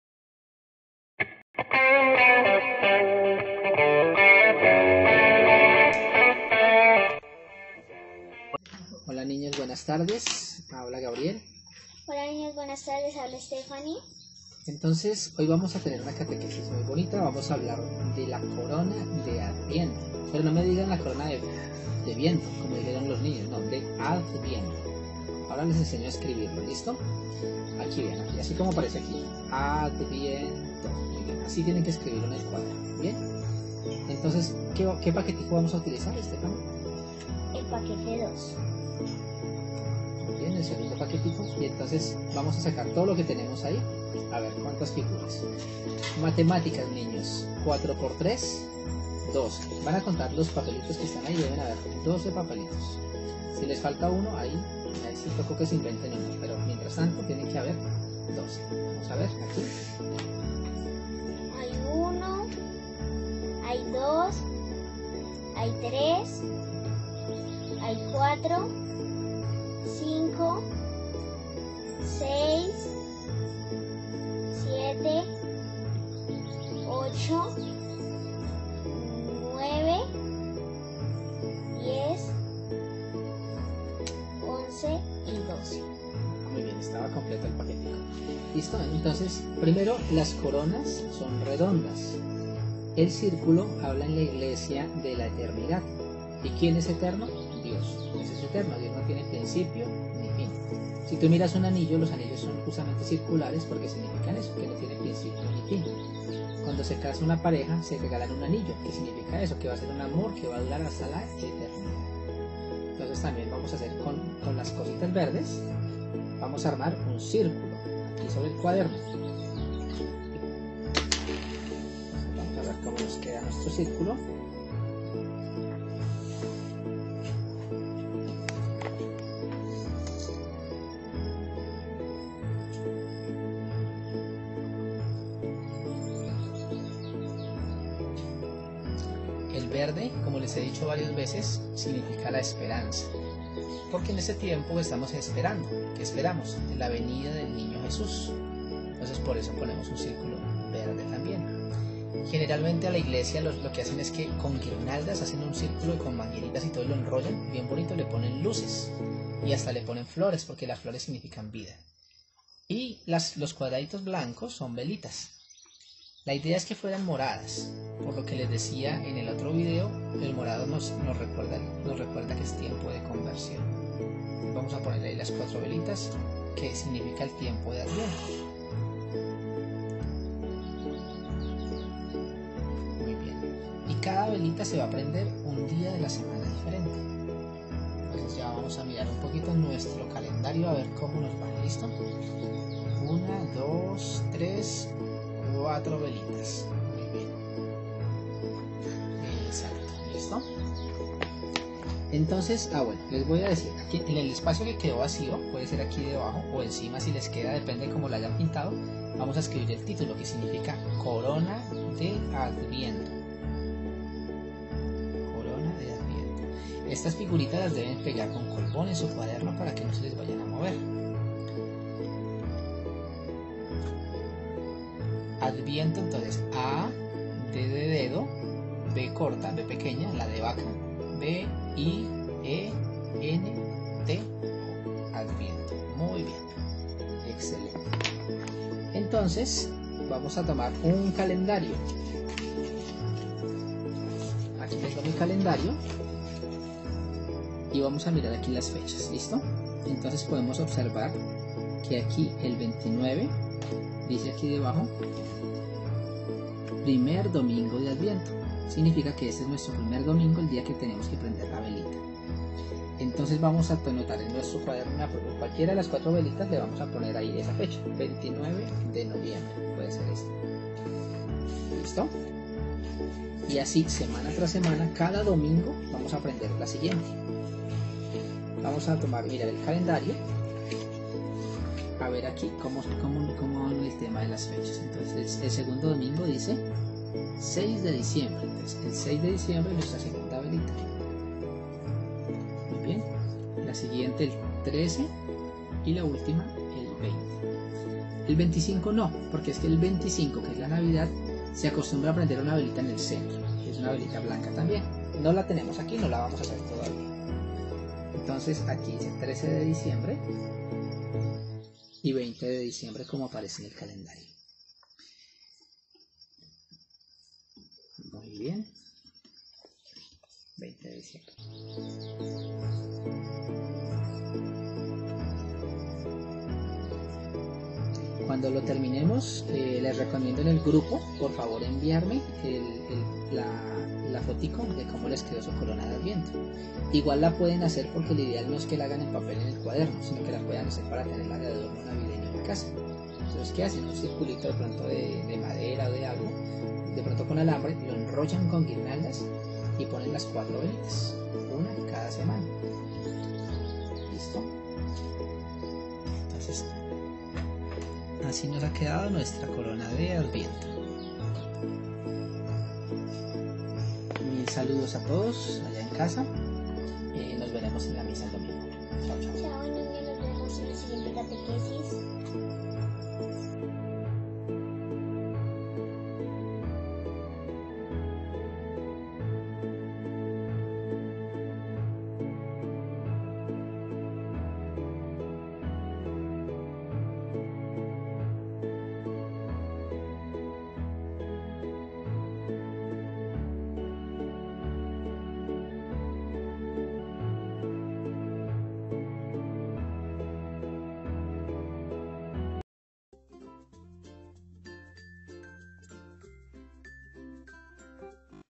Catequesis 3 - La Corona de Adviento
Hola Amigos
Hoy hablaremos de la corona de Adviento.

Te comparto el link para que descargues el archivo, lo imprimas y recortes para armar en tu cuaderno.